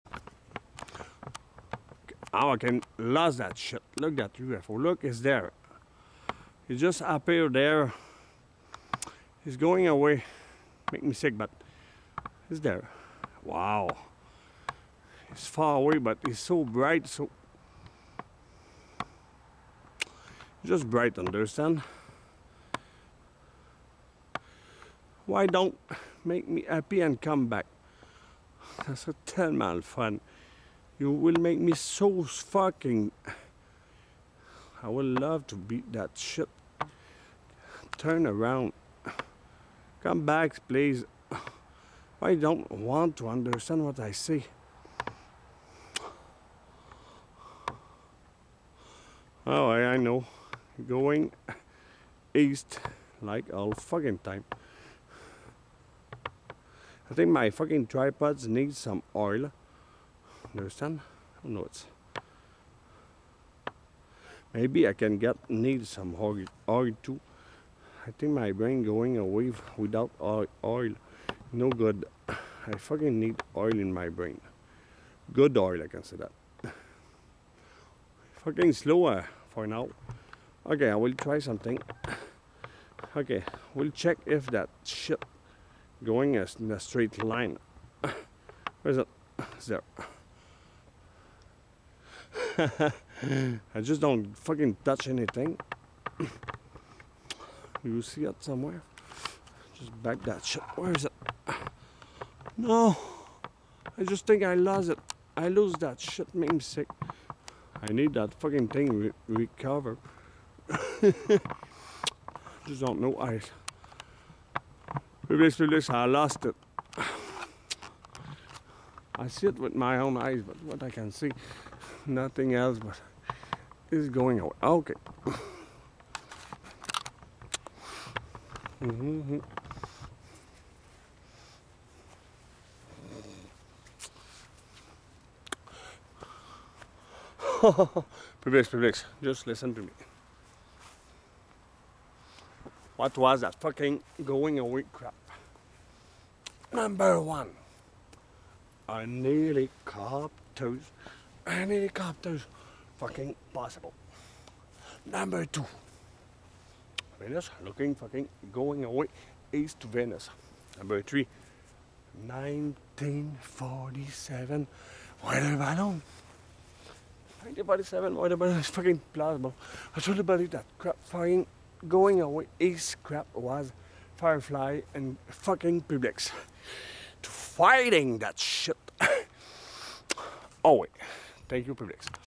UFO IN MONTREAL MAY 27 2013
AT 1:45 AM   UFO    APPEAR AWAY WHEN I FUCK IT...     WEST  TO   EAST